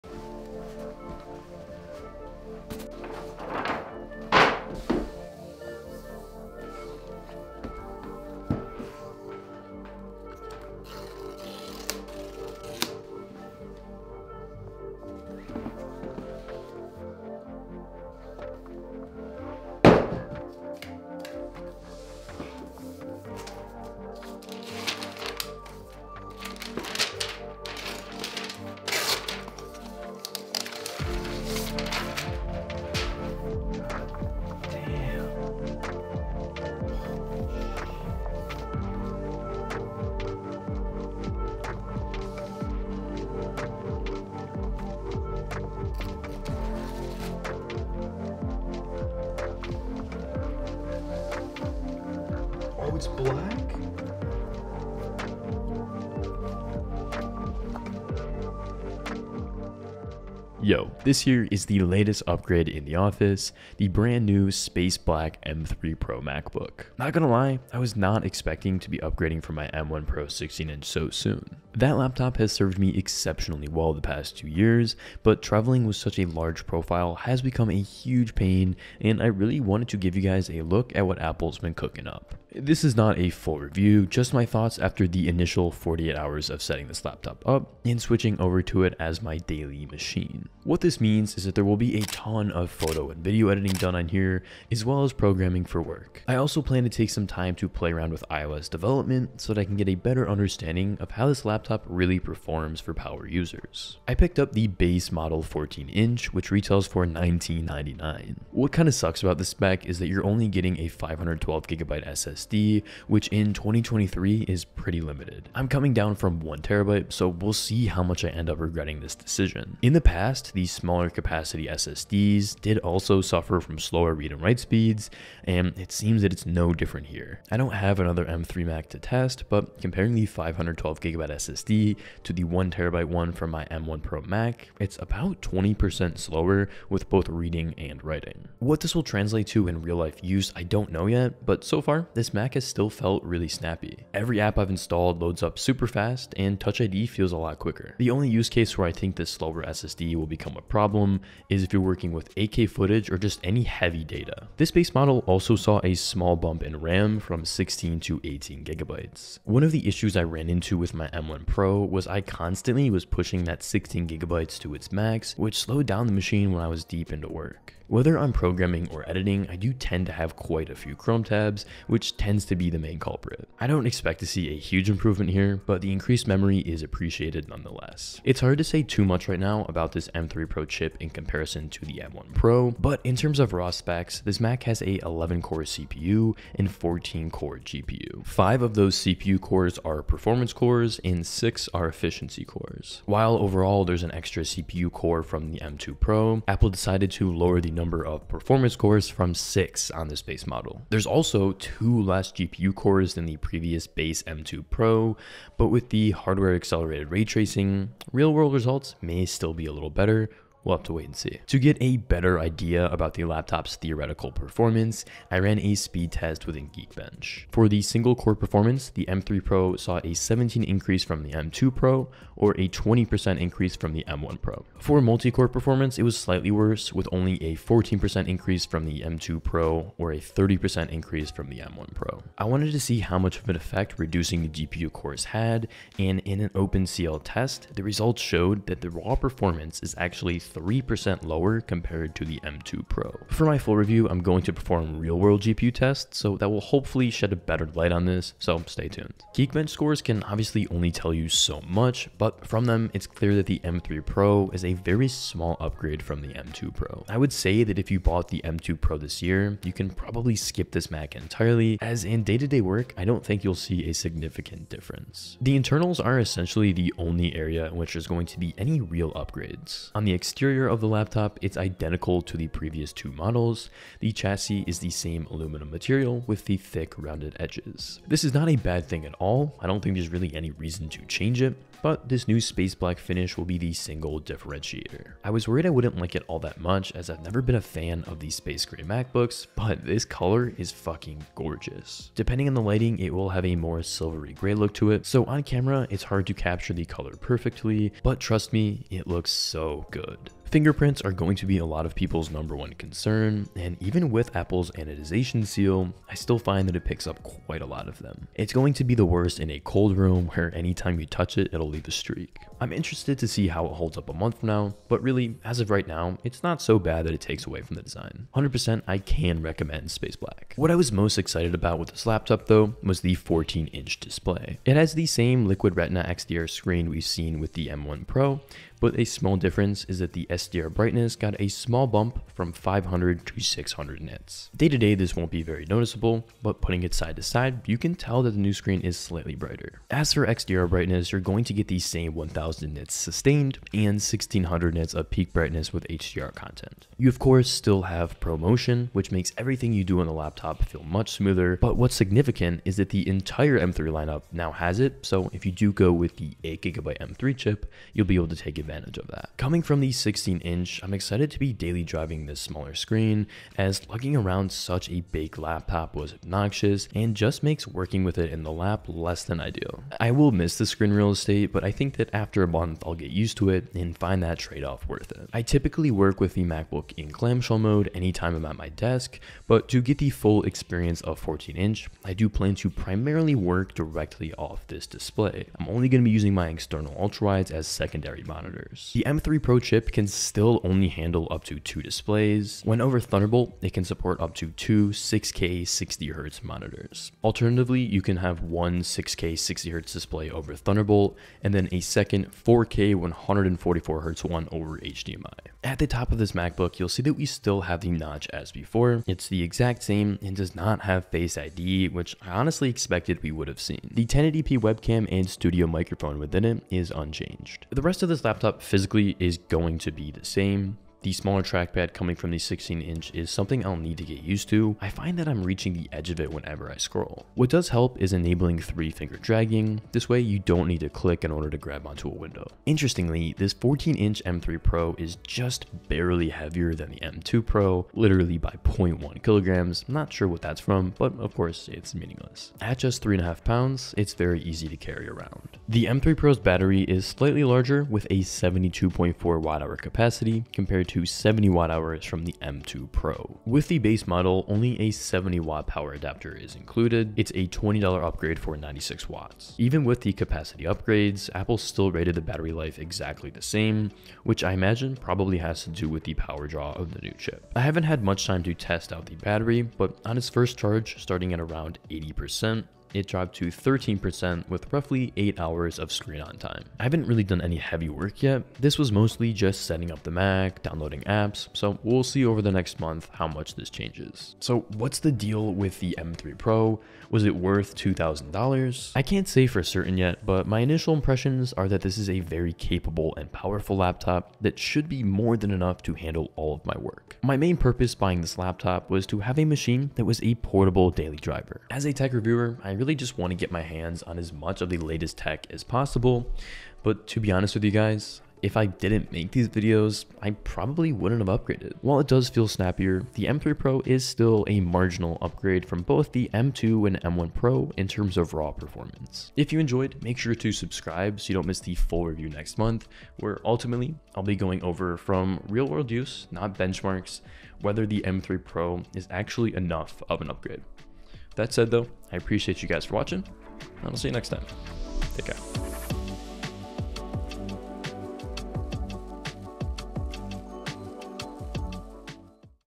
MacBook Pro M3 Pro - Base 14" Impressions
This is the NEW Base Model M3 Pro MacBook Pro 14" in Space Black. In this video I give my initial impressions after my first 48 hours with the laptop. I'll be daily driving it over the next month, mainly using it for video editing & programming (web & iOS development)

To those of you that are reading this, I appreciate the time you take out of your day to watch my videos. Thank you, from the bottom of my heart, for supporting and believing in me.